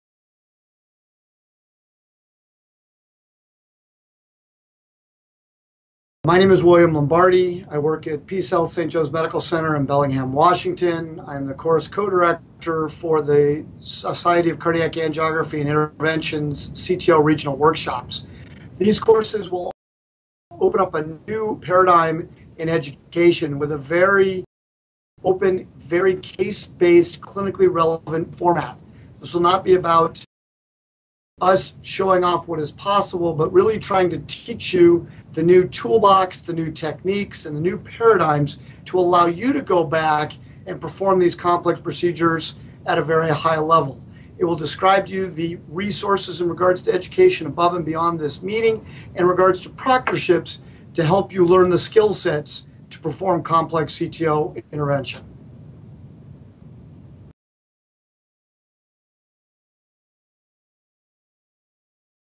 CTO Regional Training Programs: Teaching Complex CTO Interventions Skillsets
William L. Lombardi, MD, FSCAI, CTO program co-chair, highlights the goals of the CTO Regional Training Programs, to teach interventional cardiologists about the new tools and techniques to perform complex CTO interventions. Learn more about the upcoming CTO training programs